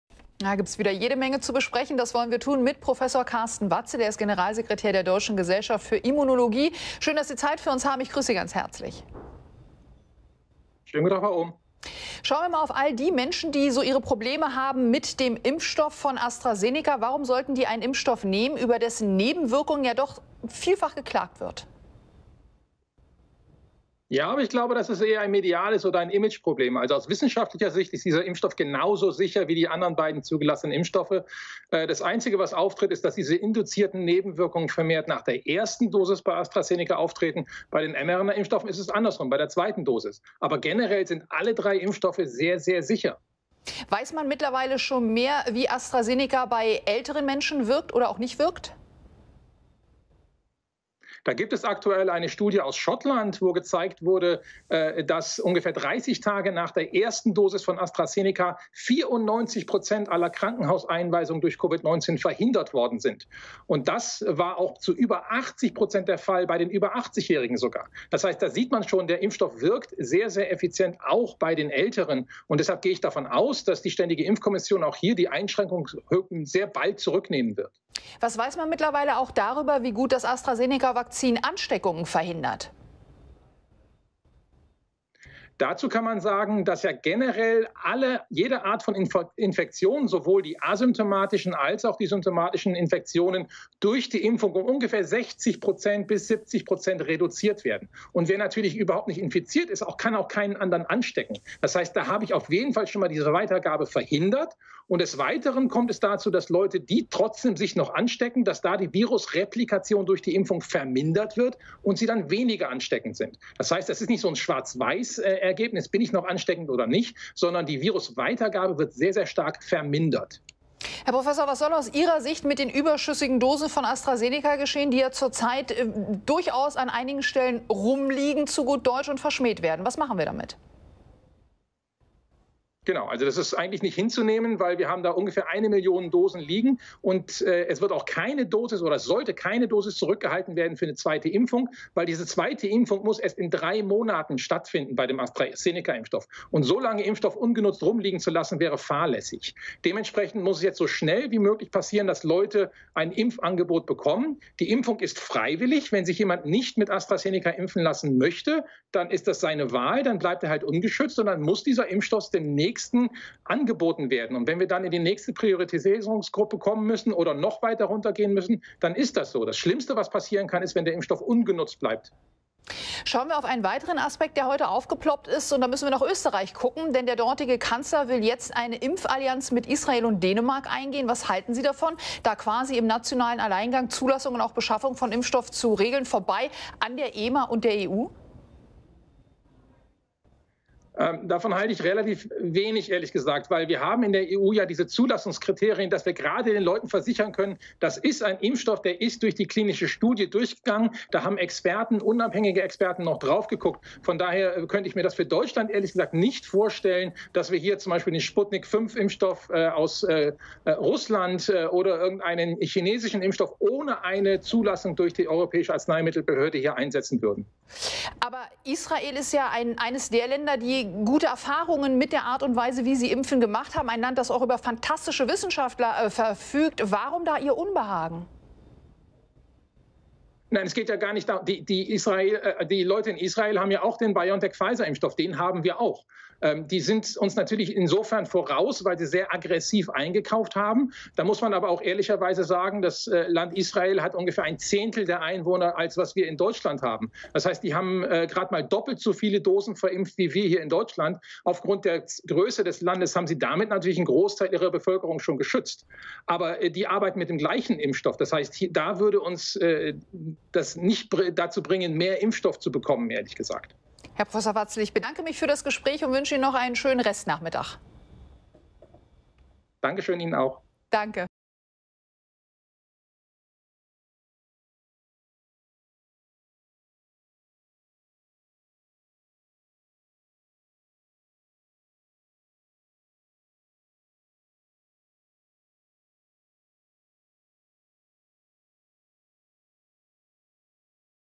MIESES IMAGE: Prof. Watzl - AstraZeneca ist ein hervorragender Corona-Impfstoff | WELT Interview
Viele Menschen wollen sich nicht mit dem Corona-Impfstoff von AstraZeneca schützen lassen. Millionen Impfdosen liegen ungenutzt in den Kühlschränken. Der Immunologe Prof. Carsten Watzl hat da eine klare Meinung. Der Impfstoff ist ein hervorragendes Mittel mit einer enormen Schutzwirkung und muss so schnell wie möglich verimpft werden.

äcorona